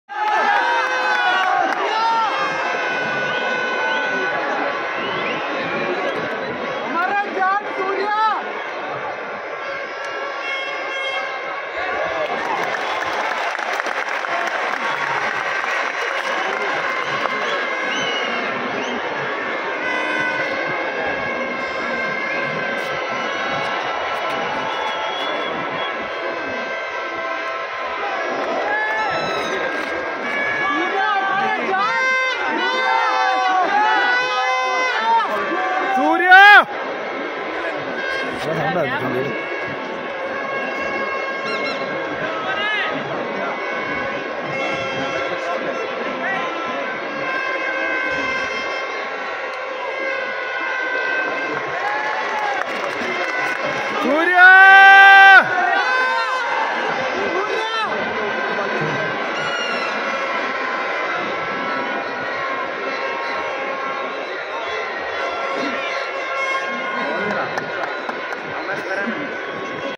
സൂര്യ കുമാർ യാദവ് കാണികളെ കയ്യിൽ എടുക്കാനും മിടുക്കനാണ് sky indvssa2ndodi cricketvideo ok
സൂര്യ കുമാർ യാദവ് കാണികളെ കയ്യിൽ എടുക്കാനും മിടുക്കനാണ്